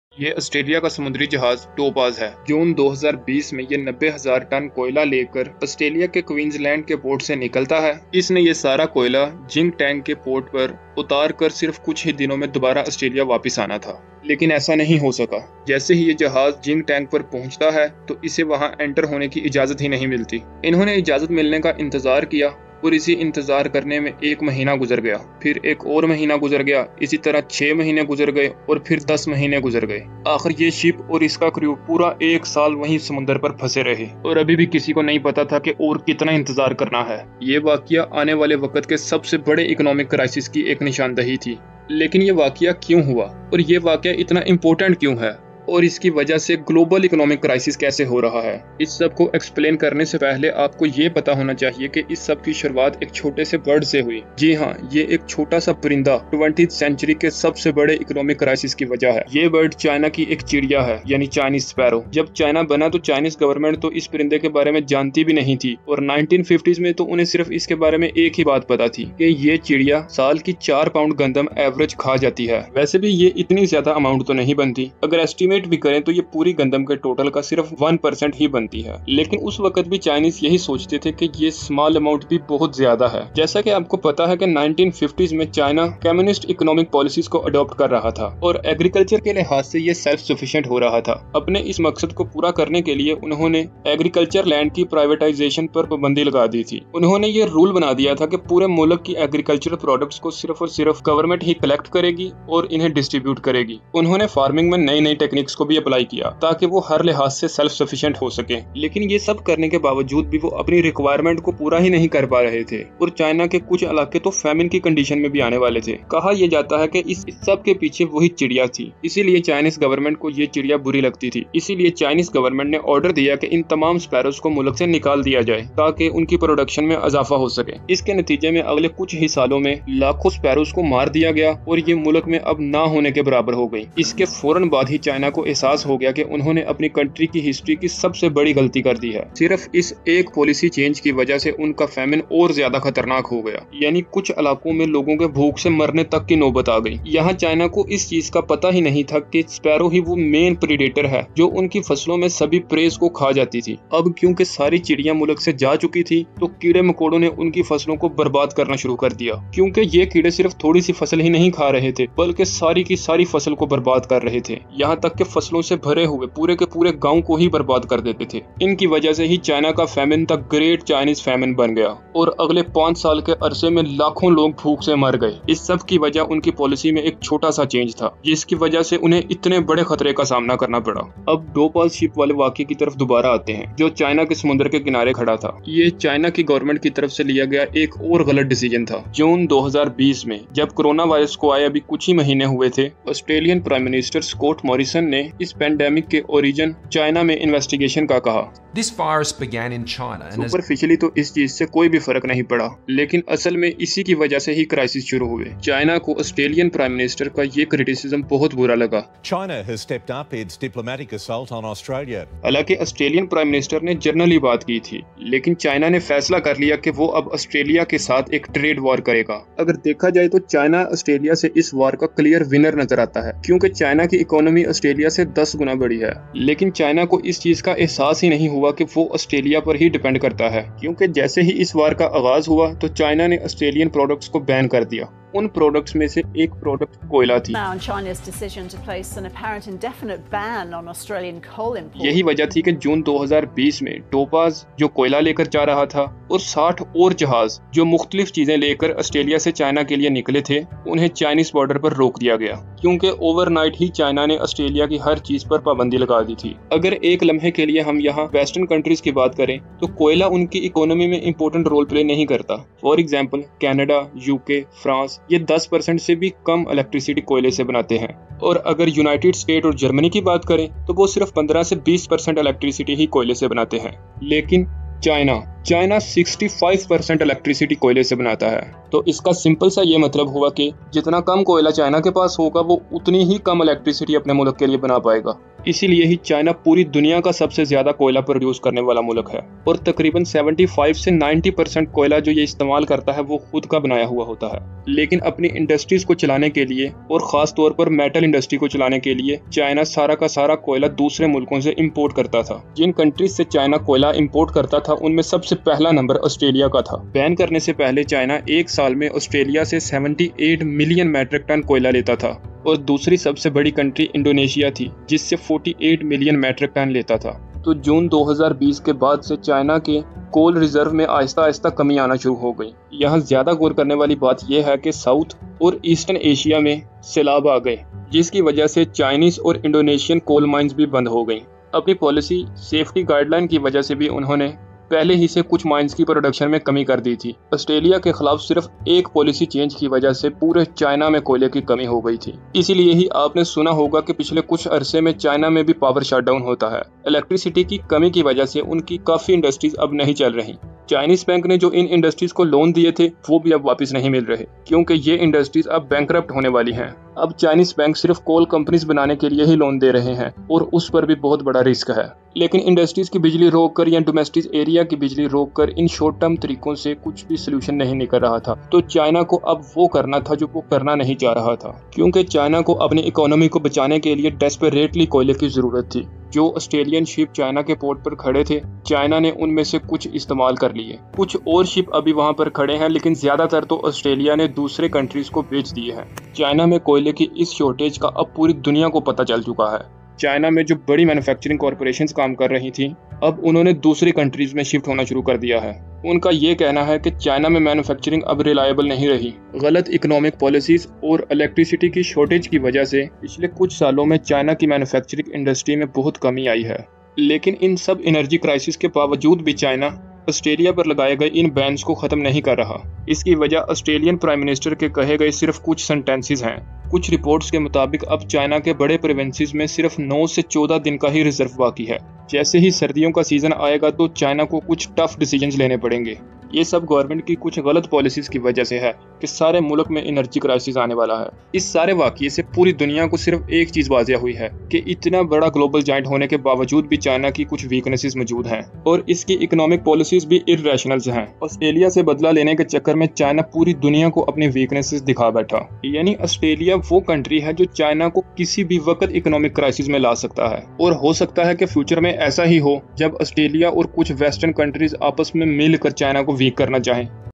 Kaise Australia China ko maat de raha hai ?
In June 2020, the Australian ship named "Topas" set sail from Queensland, Australia, carrying 90,000 tons of coal bound for the port of Jingtang, China. However, upon reaching its destination, the ship was denied entry into the Chinese port, leaving it stranded at sea for nearly a year. This event foreshadowed a significant economic issue that is currently impacting the world.

This crisis traces back to the 1950s when the Chinese government targeted sparrows, which consumed a small amount of grain annually. In an attempt to boost food production during a national famine, the government ordered the extermination of sparrows, not realizing their role in pest control. With the absence of sparrows, crop-eating pests, particularly locusts, swarmed the country, exacerbating the famine, resulting in millions of deaths.

Fast forward to modern times, Australia's call for an investigation into the origins of the pandemic triggered strained relations with China. This strained relationship, intertwined with historical policy decisions, became the precursor to a contemporary global crisis.

This narrative emphasizes how seemingly small policy changes and impulsive economic decisions can have far-reaching consequences, shaping significant crises with both historical and modern-day relevance.